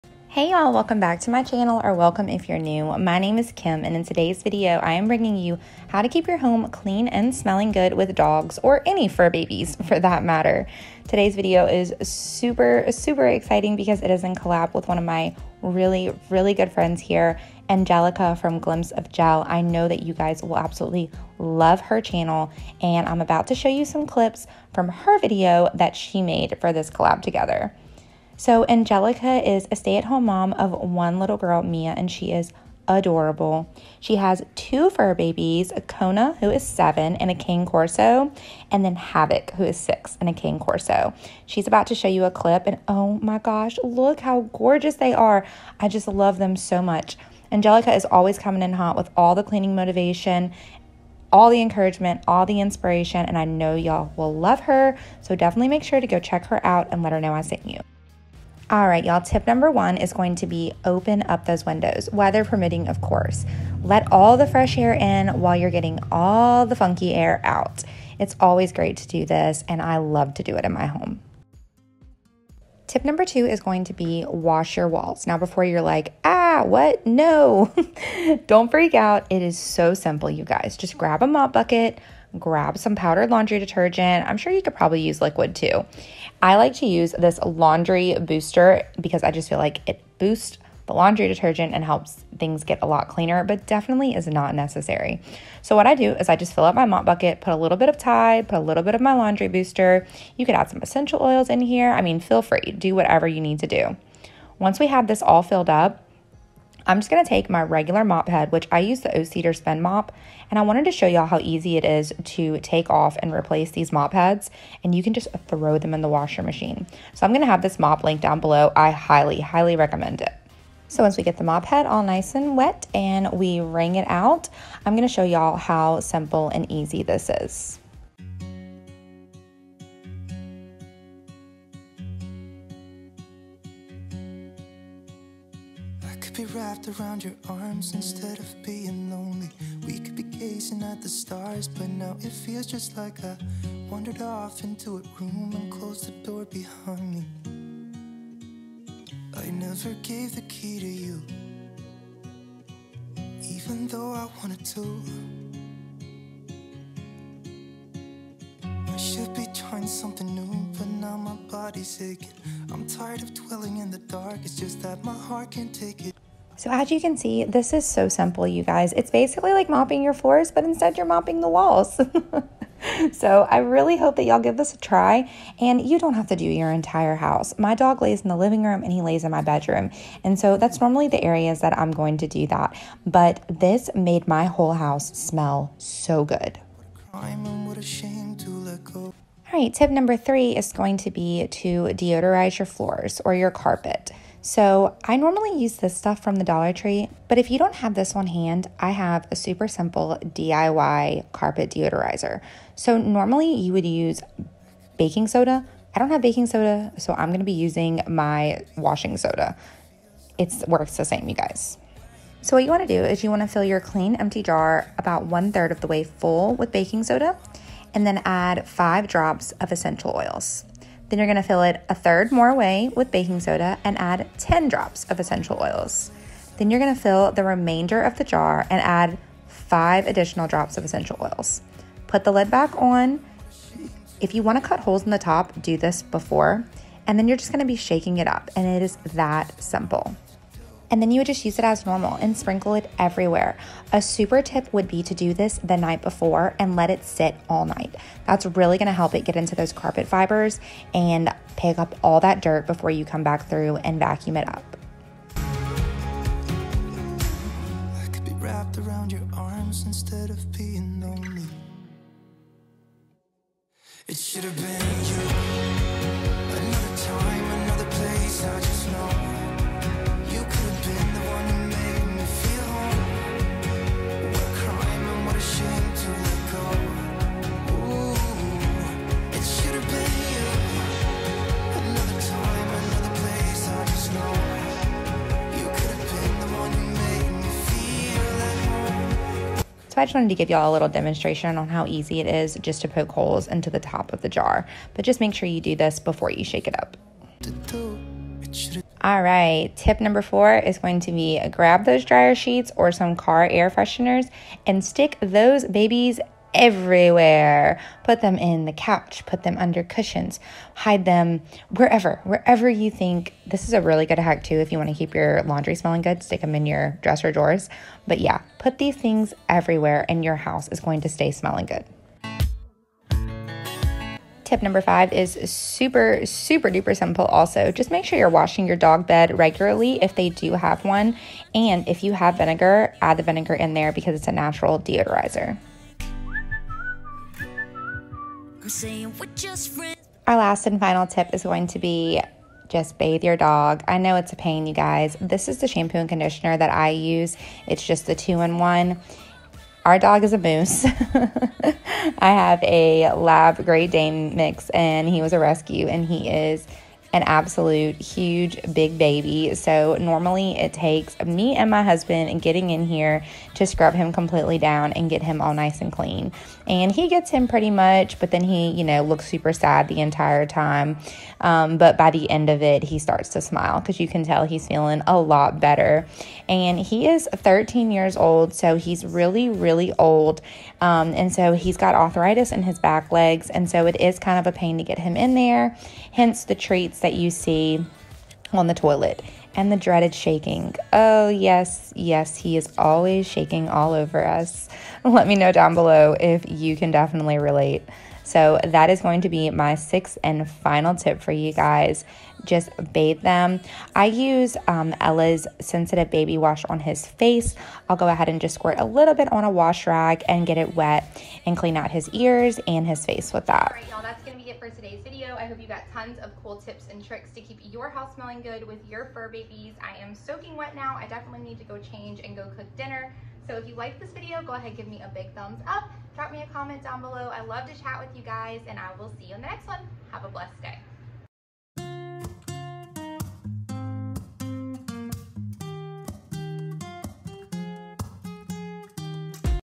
GETTING RID OF DOG ODOR | 6 WAYS TO MAKE YOUR HOUSE CLEAN AND SMELL GOOD WITH DOGS | CLEAN WITH ME
GETTING RID OF DOG ODOR | 6 WAYS TO MAKE YOUR HOUSE CLEAN AND SMELL GOOD WITH DOGS

Hey y’all! 💕 I’m so excited for today’s video because it’s in Collab with friend Angelica from Glimpse of Gel here on Youtube! 😍 I know y’all will love her so make sure to go check out her channel when you’re done watching my video and let her know I sent you! 🤗

💕My favorite mop:

💕My Vacuum: